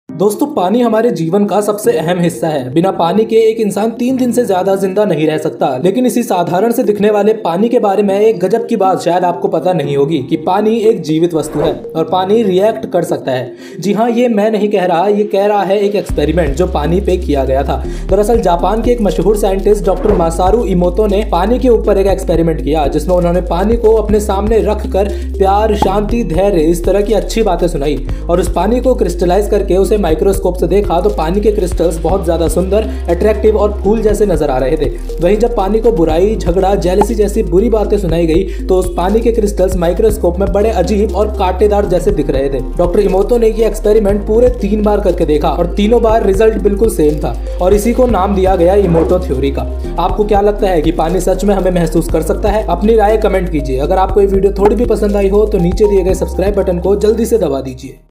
पानी एक जीवित वस्तु है और हमे सुन सकता है 😨 Masaru Emoto Water Experiment
Dr. Masaru Emoto of Japan performed an experiment on the water in which he said some good words in front of water like love, peace, etc. and crystalize the water and when he checked the water crystals in a microscope, they were looking like flowers and when he said bad words to water then the water turned into strange thorns like shape.

How do I make videos?
MIC: Boya
AUDIO RECORDING: Mobile
AUDIO EDITING: Audacity
VIDEO EDITING: Cyberlink PowerDirector (Mobile)